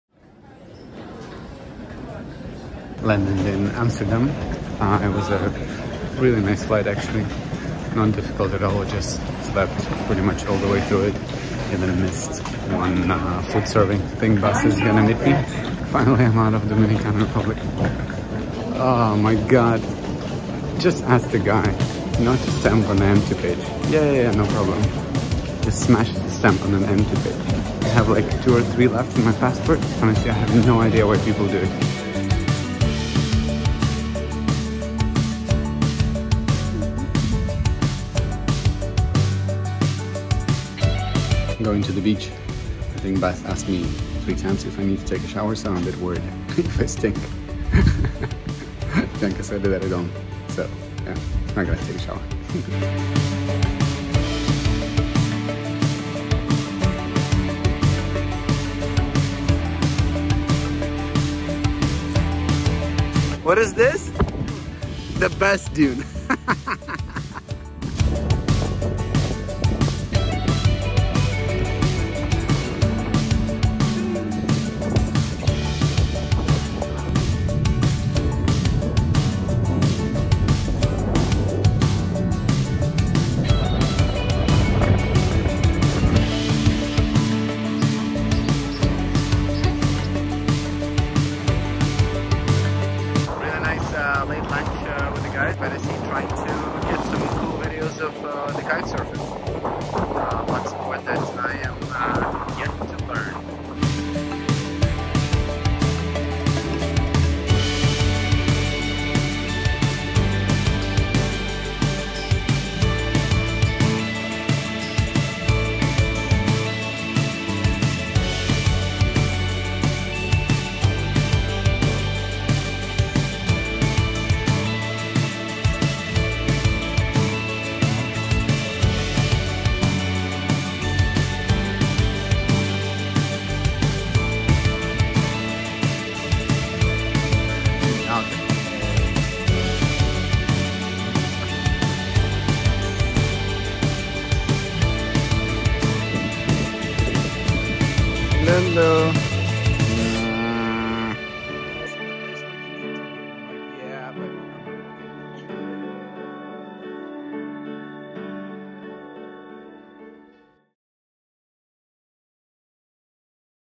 Netherlands VLOG#2
Back to Leiden to see Bas and Bianca🥰